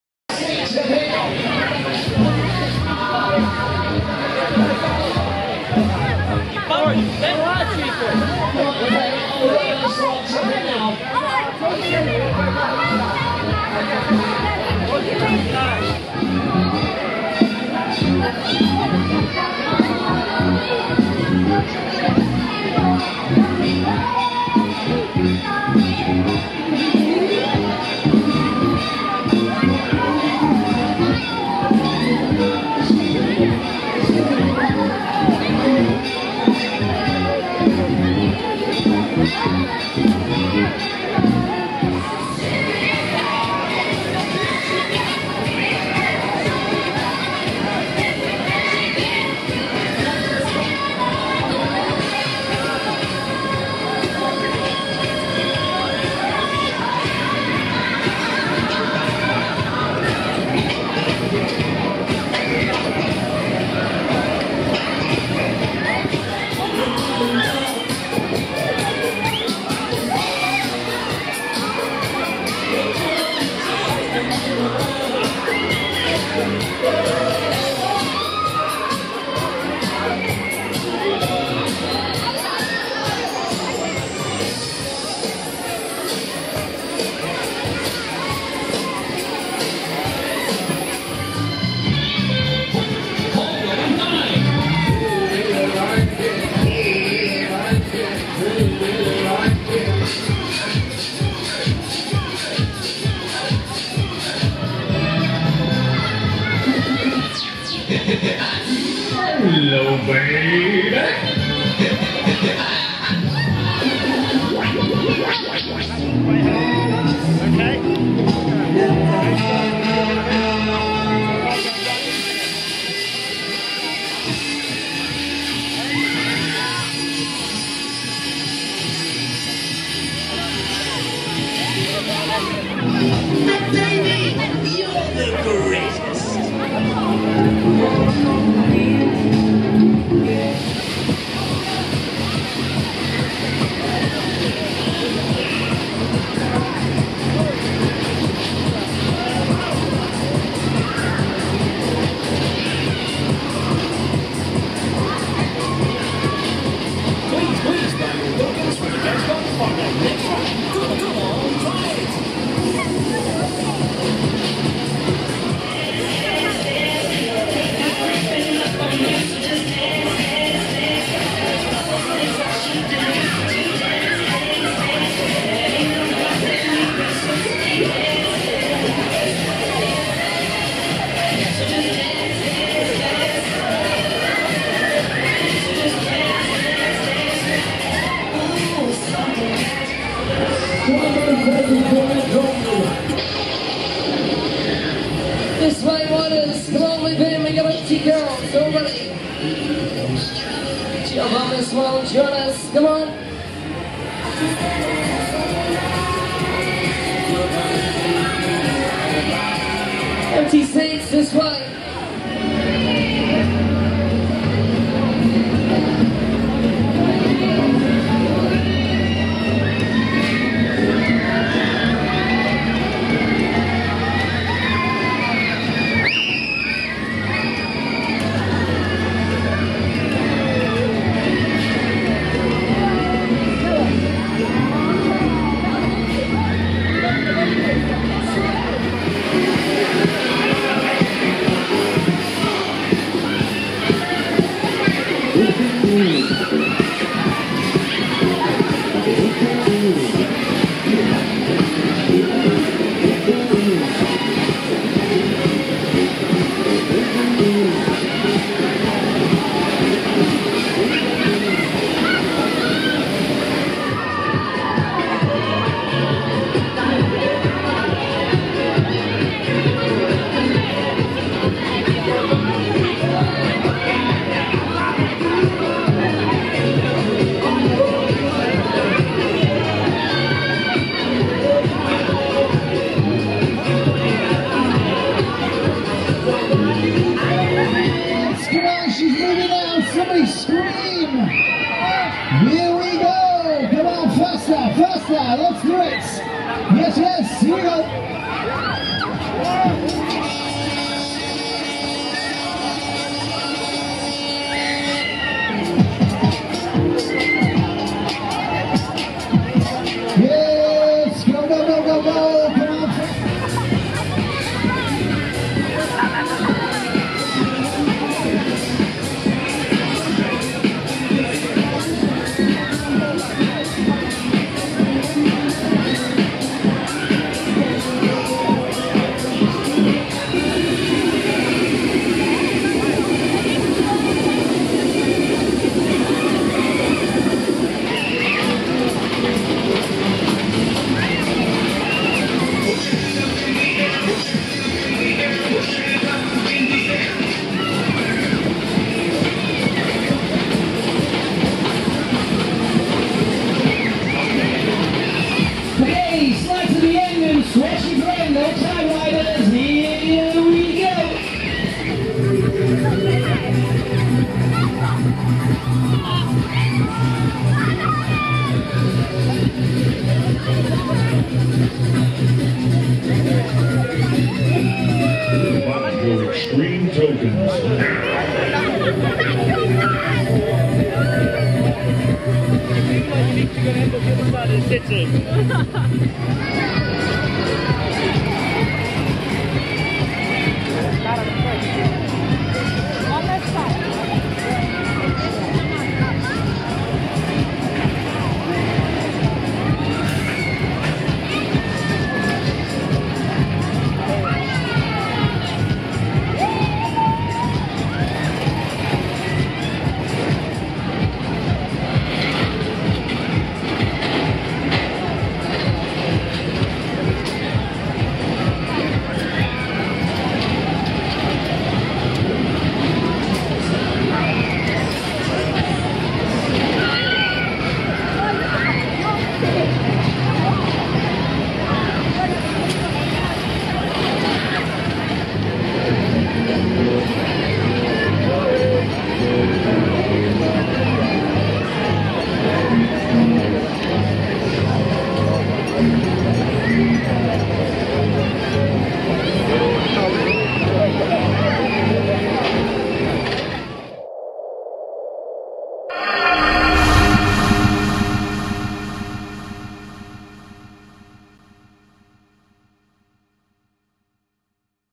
Nottingham Riverside Festival 2019
Held on the 2nd - 4th August on the Victoria Embankment of Nottingham with James Mellors at lessee.